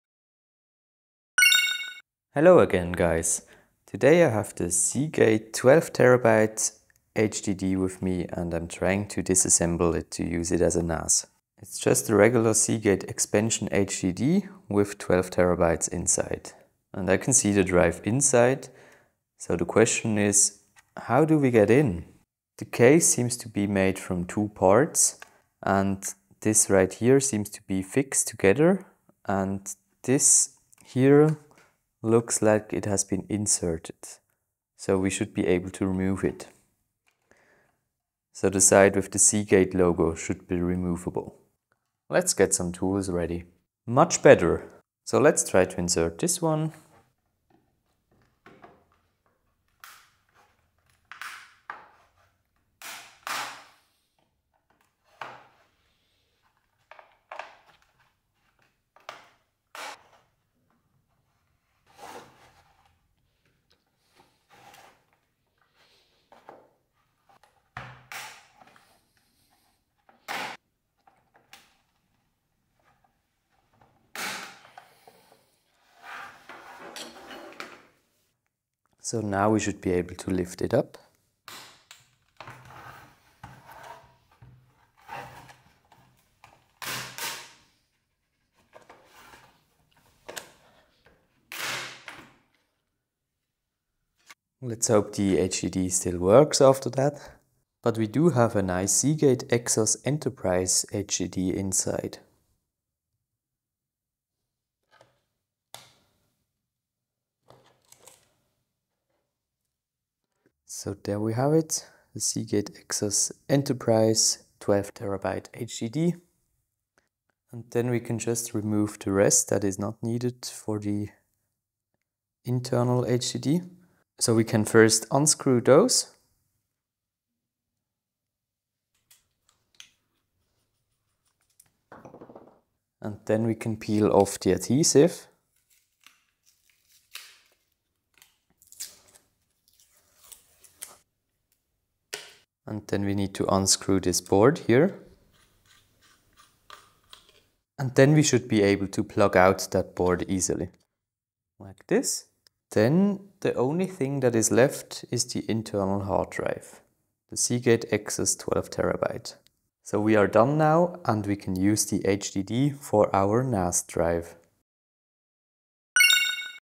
Seagate 12TB HDD Teardown: Exposing the Exos X18 Inside!
We disassemble the massive 12TB Seagate Expansion HDD to unveil the powerhouse within - the Exos X18 Hard Drive. Perfect for those looking to repurpose high-capacity drives for NAS projects or simply curious about what's inside these storage giants. Watch as we break it down, step by step.

Seagate Expansion HDD
Seagate Exos X18

Got questions or thoughts about the Seagate Expansion HDD or the Exos X18?